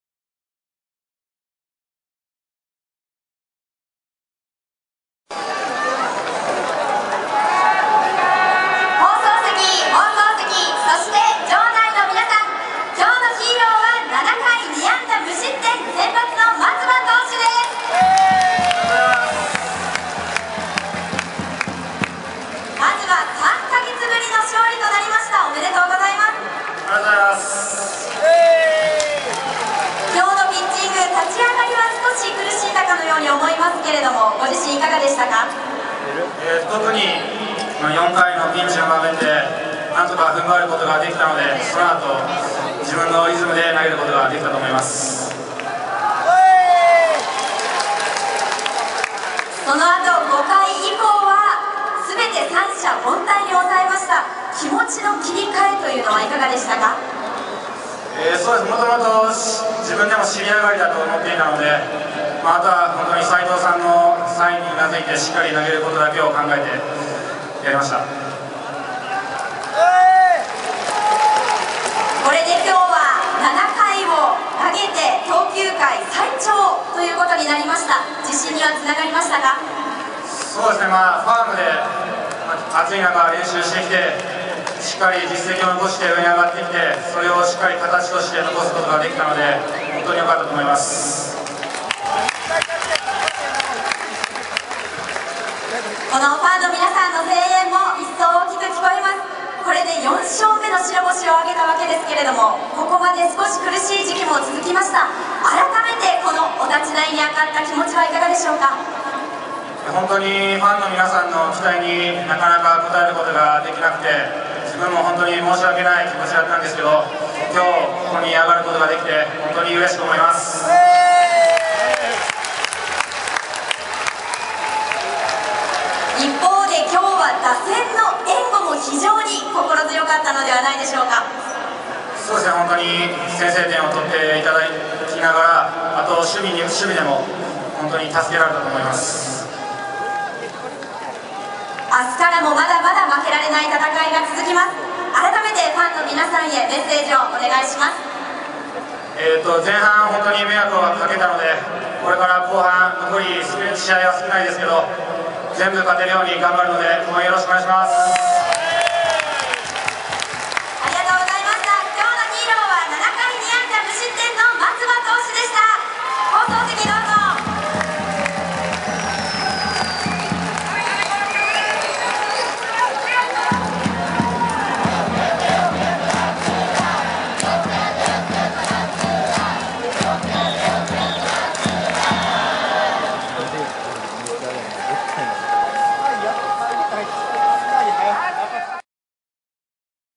130827 オリックス 3ヶ月ぶりの勝利で4勝目 松葉貴大 投手 ヒーローインタビュー
2013年8月27日（火）京セラドーム大阪でのオリックス・バファローズvs東北楽天ゴールデンイーグルス。4-0でオリックス・バファローズが勝利。7回、被安打2、無失点で好投した先発の松葉投手がヒーローインタビューに登場。